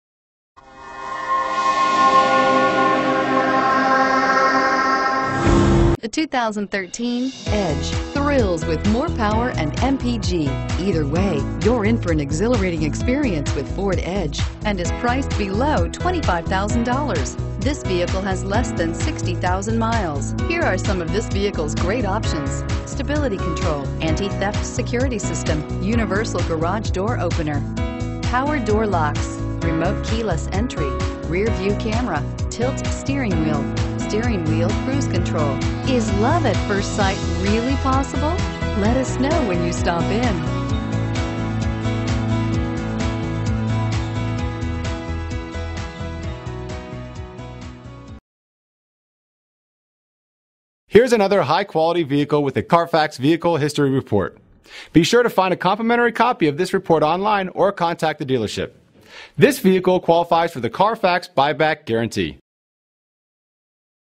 2013 FORD EDGE Irving TX DBA52499
Looking for the right 5dr? Today could be your lucky day. With 59739 miles, this Black, 2013 FORD EDGE equipped with Automatic transmission could be yours. Request more information and set up a test drive right away.